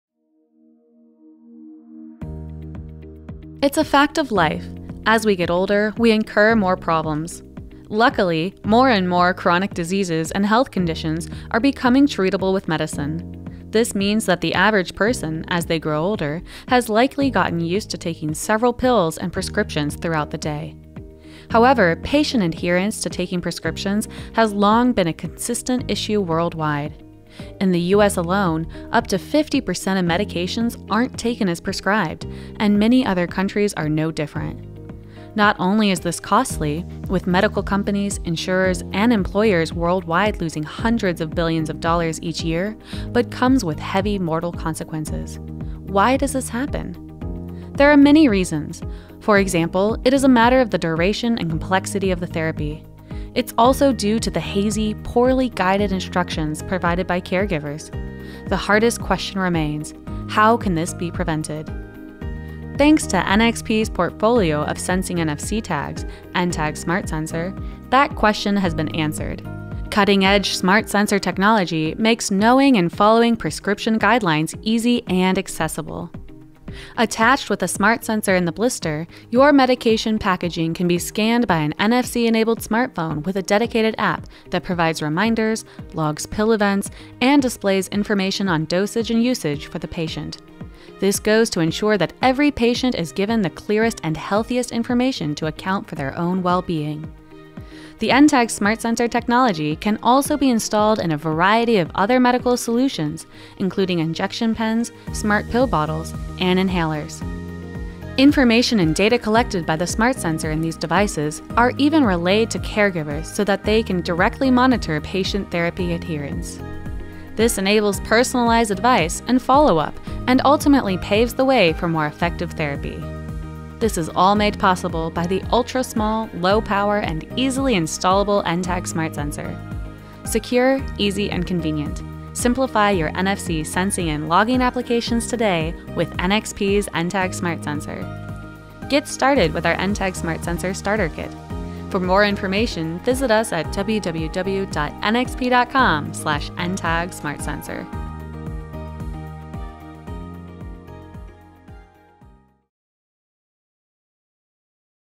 Smart Patient Adherence with NXP's NTAG SmartSensor
In the developed world, less than 50% of chronic patients take their medication as prescribed. The World Health Organization (WHO) believes that increasing medication adherence has a more significant impact on the health of the population than any improvement of medical treatments.

NXP’s NTAG SmartSensor enables intelligent packaging solutions that remind and encourage patients to take their pills.